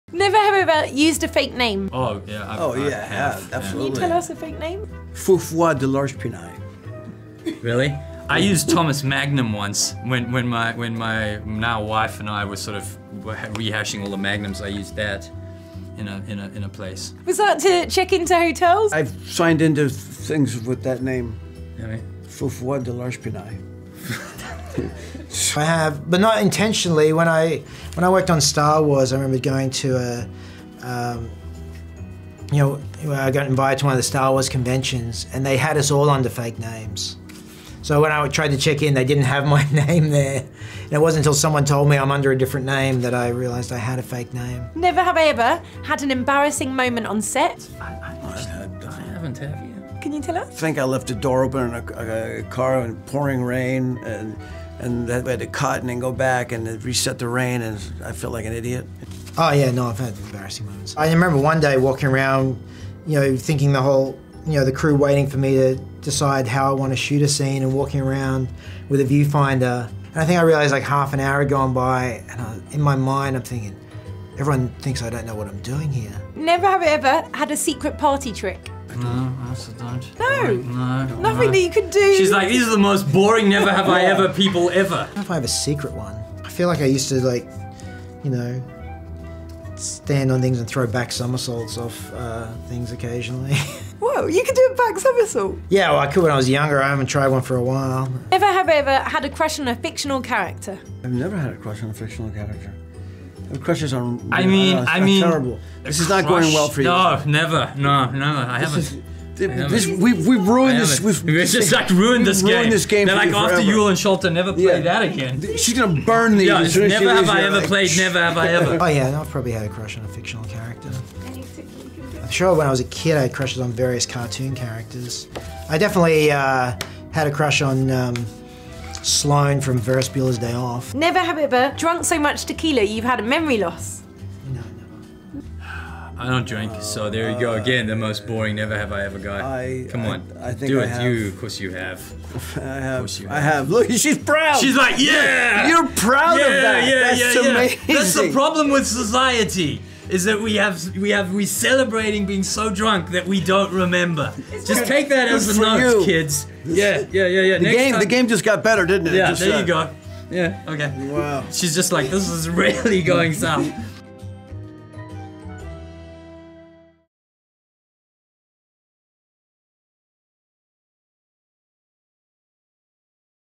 Gringo Cast Play Never Have I Ever | MTV Movies
Yul Vazquez, Sharlto Copley and Nash Edgerton play a fun game of Never Have I Ever. Find out the cast's fake names, embarrassing moments & whether or not anyone's been so drunk that they've had memory loss.

Cast/ director list - Matthew Stone, Anthony Tambakis, Charlize Theron, Amanda Seyfried, Joel Edgerton , Kenneth Choi , Thandie Newton , Michael Angarano , David Oyelowo , Harry Treadaway , Paris Jackson